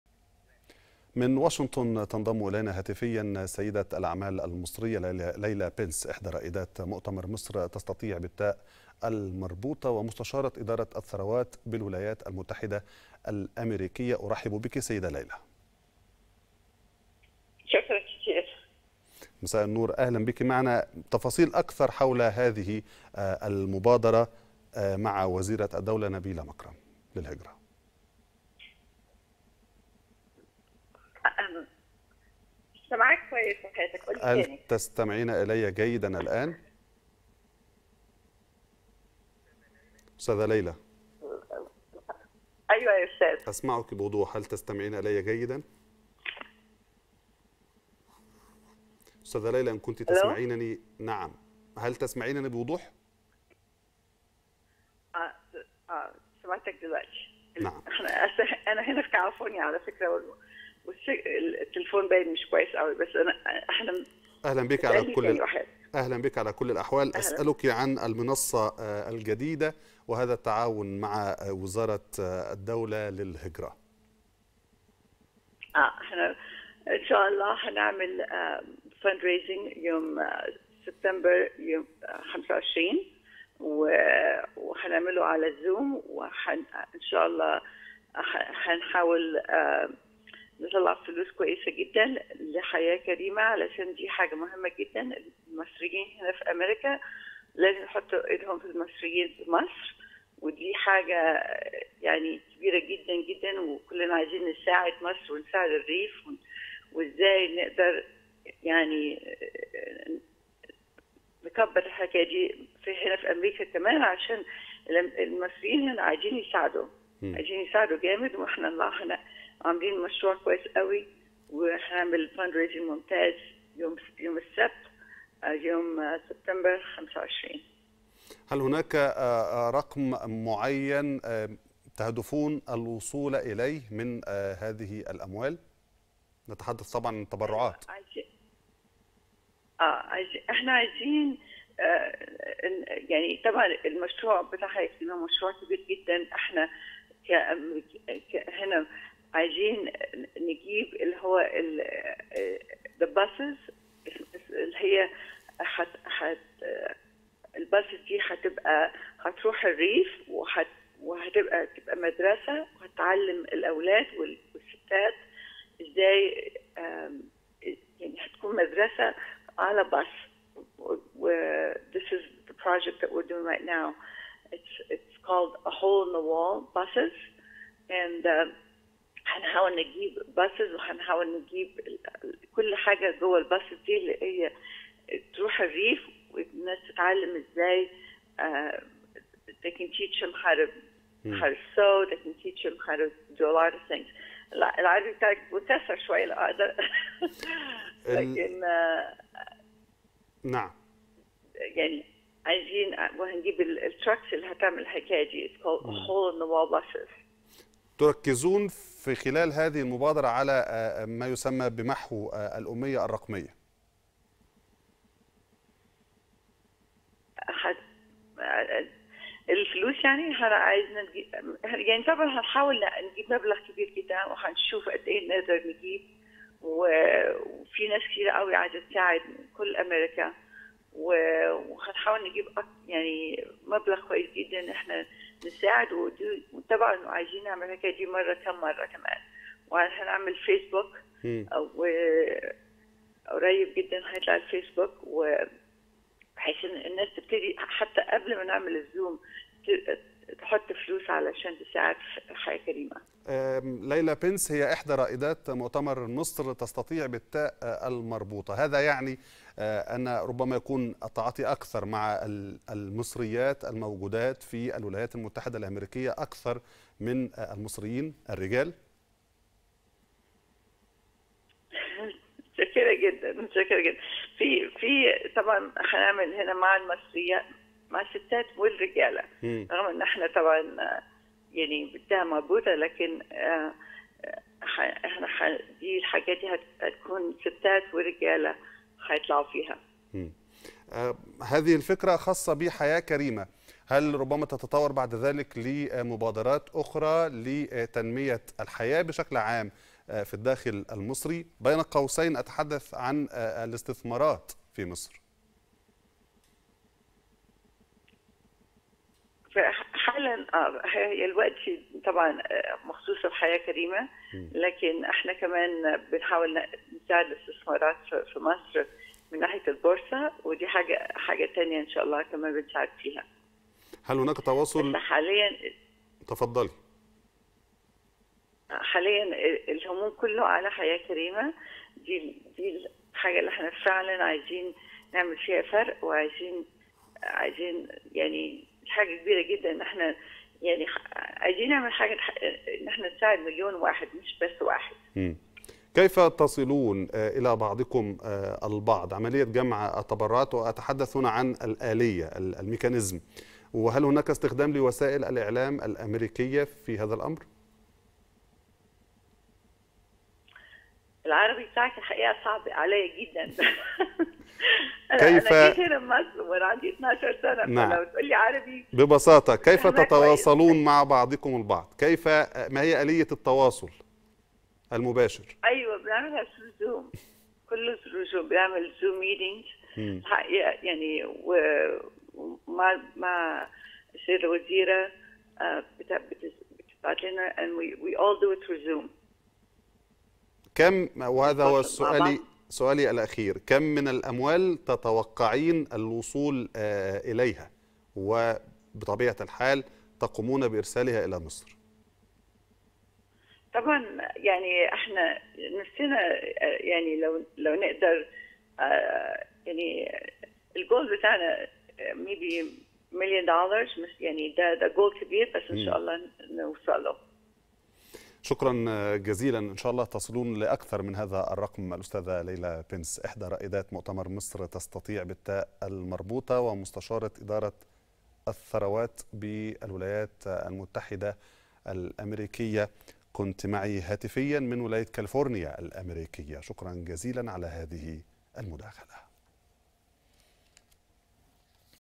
مستشارة إدارة الثروات بالولايات المتحدة تكشف تفاصيل تعاونها مع وزارة الهجرة في "مبادرة حياة كريمة"
أخبار_TeN - شاهد تعليق ليلى بنس مستشارة إدارة الثروات بالولايات المتحدة على مبادرة «حياة كريمة» وتفاصيل تعاونها مع وزارة الهجرة